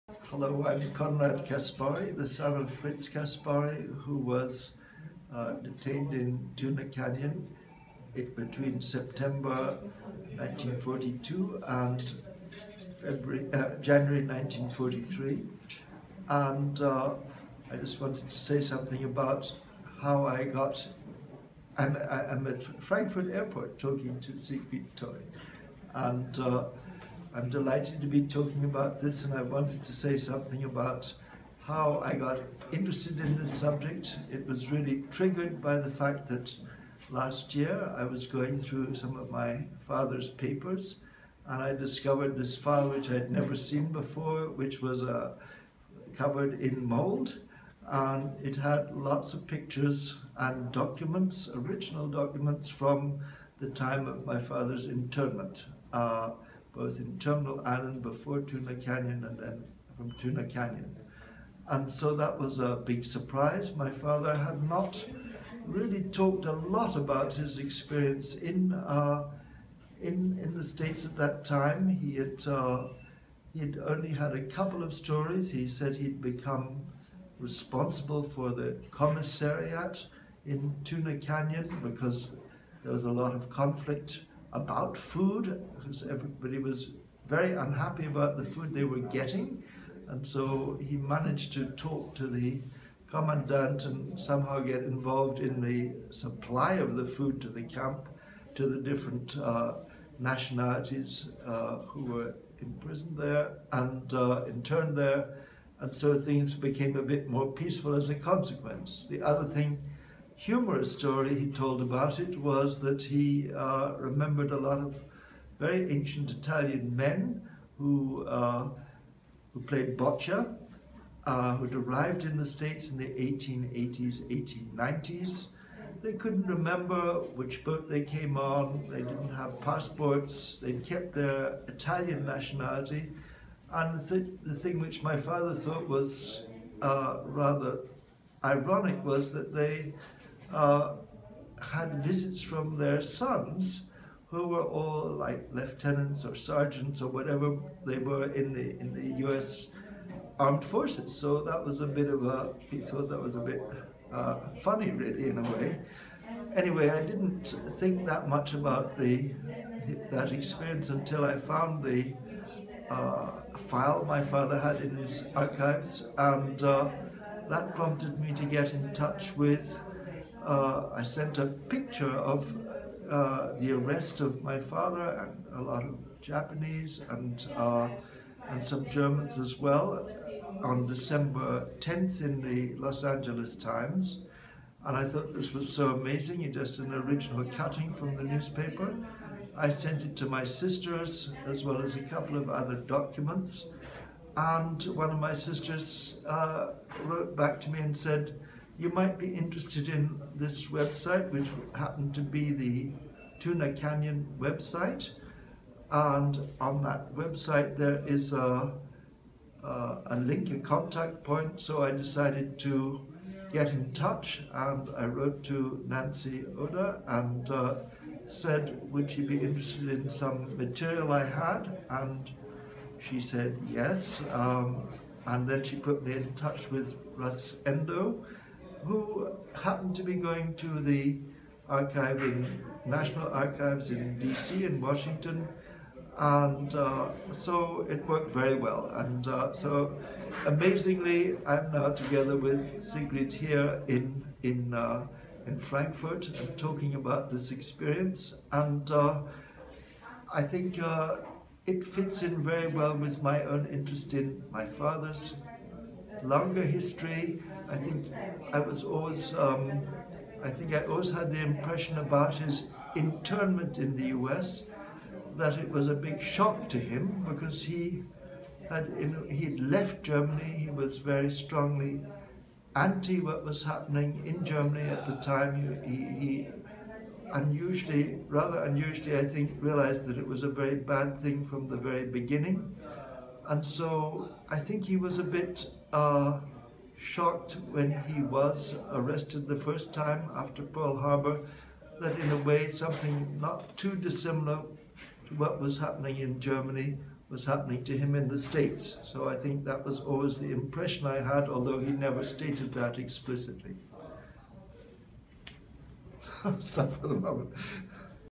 Conrad Caspari
Conrad speaking about his father, Fritz Caspari